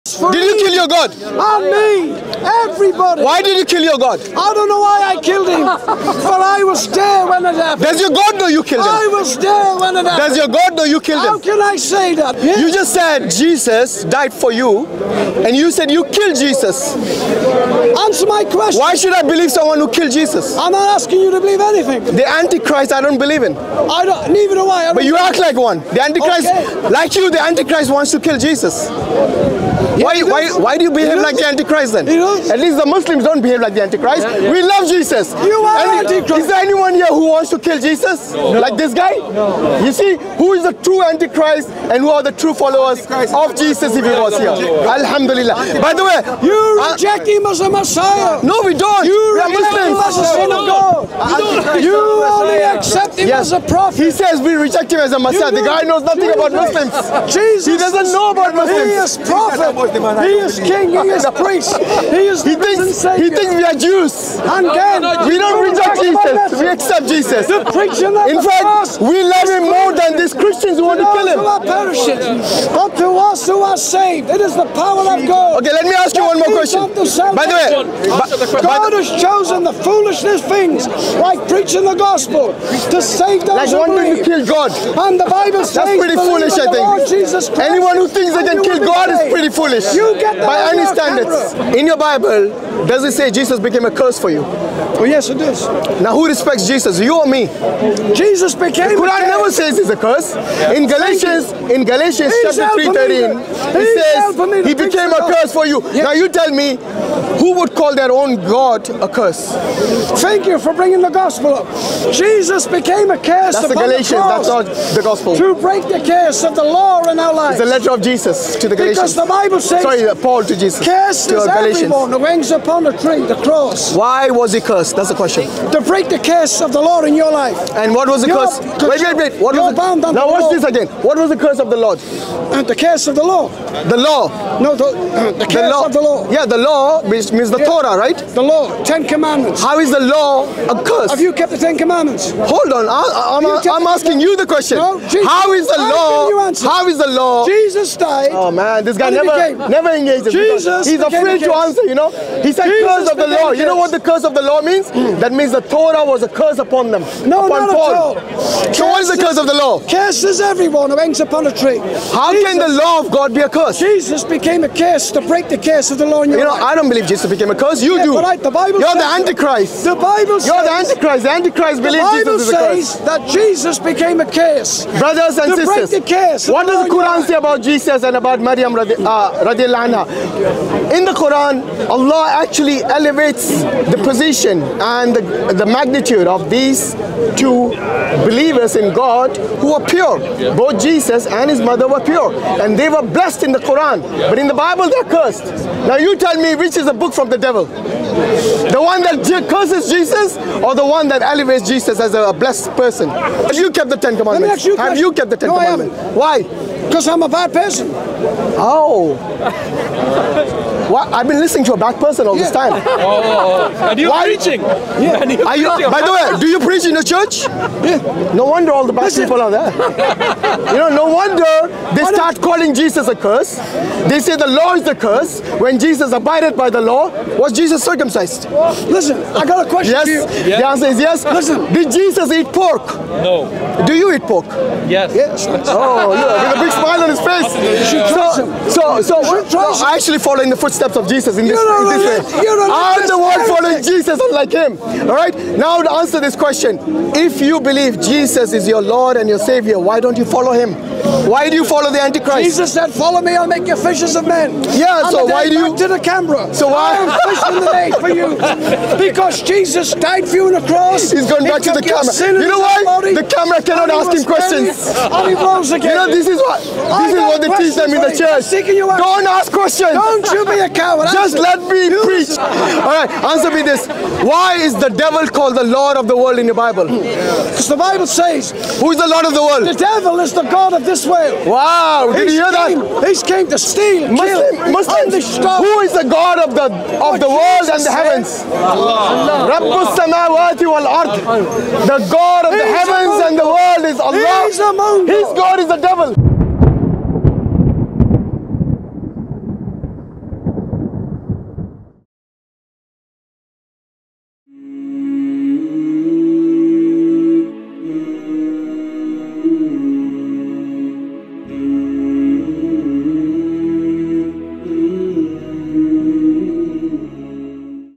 Bible Kata Syaitan Adalah Tuhan? Saat Kristian Sedar Dia Mengikut Dajjal!
Bro Hashim berdialog dengan seorang Pastor Kristian tentang ajaran-ajaran Kristian yang bercanggah dengan ajaran Jesus malah di dalam Bible juga disebut bahawa Syaitanlah Tuhan di dunia ini yang memesongkan jalan yang benar. Lihat bagaimana bro Hashim membuatkan Pastor Kristian ini mula berfikir bila dibentangkan hujah-hujahnya yang padu.